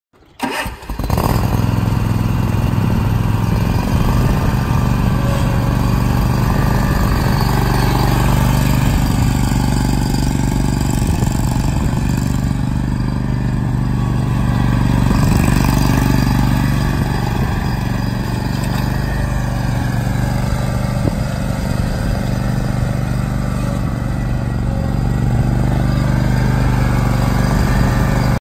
Hustler Raptor Super Duty 60" Zero Turn Mower--Sells June 25, 2019
Check out this great mower which will sell in NW IA for the Kenny Van Wyhe Retirement auction on June 25, 2019!